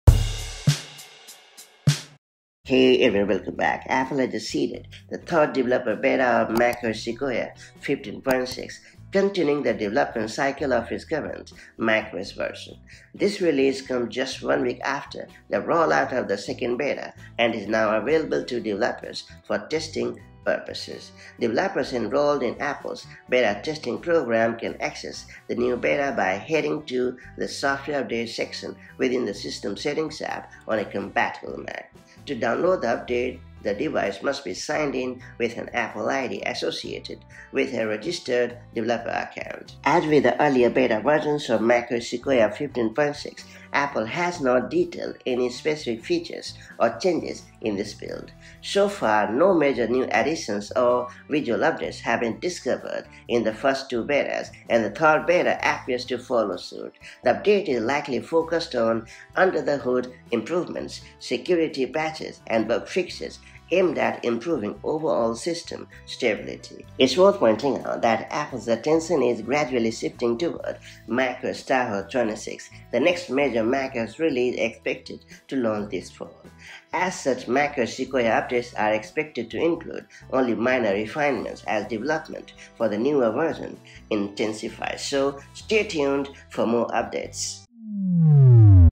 macOS Sequoia 15.6 Beta 3 Released: What's New?
Apple has just dropped macOS Sequoia 15.6 Beta 3 for developers! In this video, we dive into everything new in this latest beta release. While major new features are typically reserved for the main macOS Sequoia (15.0) and future macOS 16 "Tahoe" updates, this beta focuses on crucial bug fixes, performance improvements, and security enhancements.